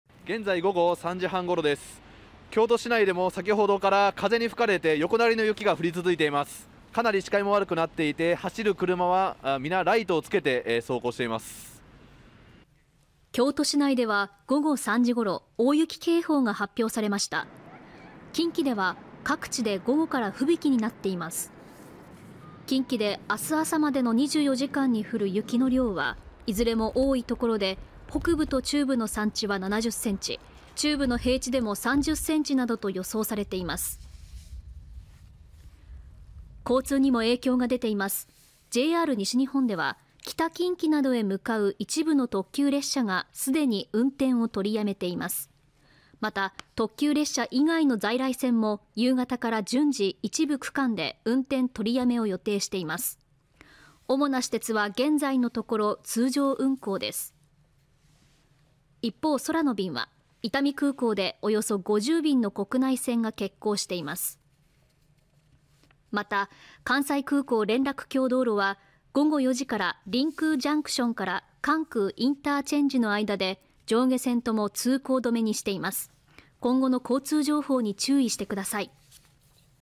京都市に大雪警報 JR西日本「きのさき」「はしだて」など一部の特急で運転取りやめも（2023年1月24日）
これから近畿地方や徳島県では警報級の大雪となるおそれがあります。

　（記者リポート　午後３時半ごろ）
　「京都市内でも先ほどから、風に吹かれて横殴りの雪が降り続けています。かなり視界も悪くなっていて、車はライトをつけて走行しています」

　京都市内では２４日午後３時ごろに大雪警報が発表されました。近畿では各地で午後から吹雪になっています。近畿で２５日朝までの２４時間に降る雪の量は、いずれも多い所で、北部・中部の山地は７０ｃｍ、北部の平地で４０ｃｍ、中部の平地で３０ｃｍ、南部の山地で３０ｃｍと予想されています。

　交通にも影響が出ています。ＪＲ西日本では２４日、北近畿などへ向かう一部の特急列車がすでに運転を取りやめています。また、特急列車以外の在来線も夕方から順次一部区間で運転取りやめを予定しています。主な私鉄は２４日午後４時時点で通常運行となっています。

　【一部運転取りやめ】
　「きのさき」「はしだて」「まいづる」・「こうのとり」・「スーパーはくと」など

　【一部区間で運転取りやめ　午後２時以降順次】
　湖西線・山陰本線・播但線・舞鶴線・福知山線・加古川線

　一方、空の便は２４日午後４時半時点で、伊丹空港で計約５０便（発・着）の国内線が欠航しています。また、関西空港連絡橋道路は２４日午後４時から、りんくうＪＣから関空ＩＣの間で上下線とも通行止めにしています。今後の交通情報に注意してください。